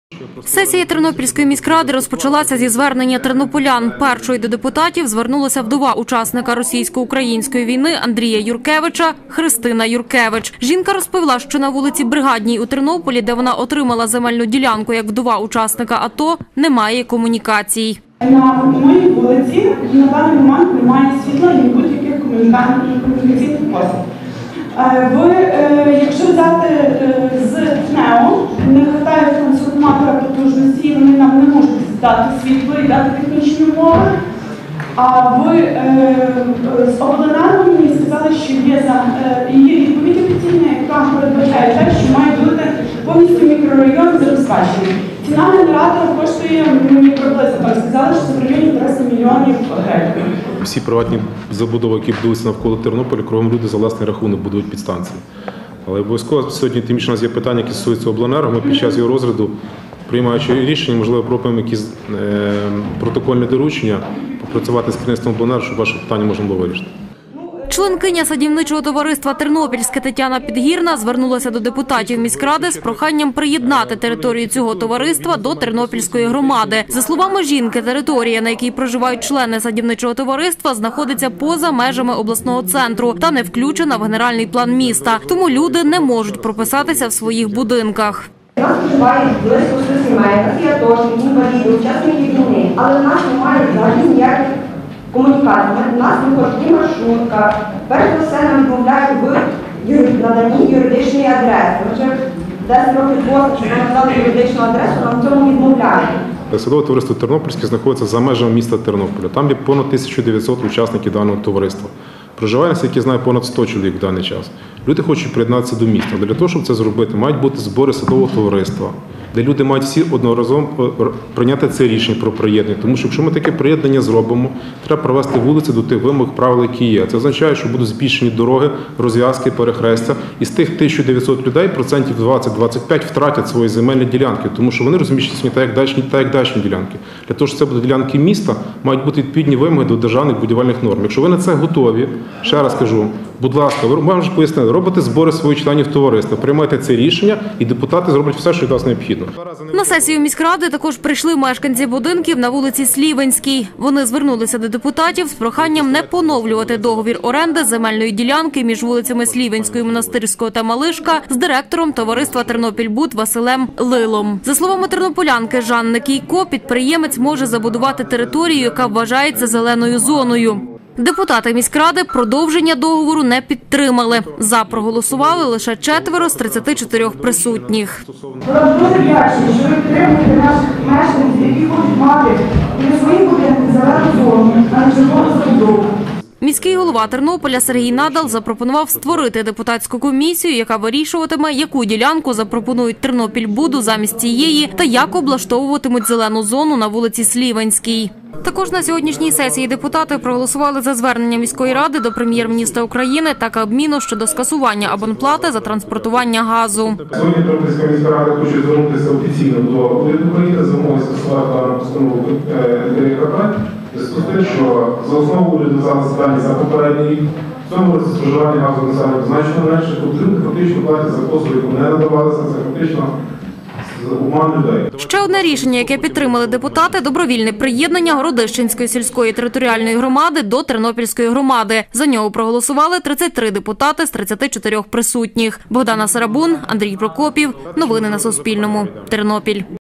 У Тернополі збережуть зелений сквер. Міськрада не проголосувала за поновлення оренди "Тернопільбуду
На вулиці Слівенській у Тернополі буде зелена зона. На сесії депутати не проголосували за поновлення договору оренди землі на цій вулиці з товариством "Тернопільбуд". Про це повідомила керівниця пресслужби міськради Мар'яна Зварич.

На сесії були тернополяни, які звернулися до депутатів, щоб вони підтримали їх та виступили за збереження скверу між вулицями Слівенською, Монастирського та Малишка.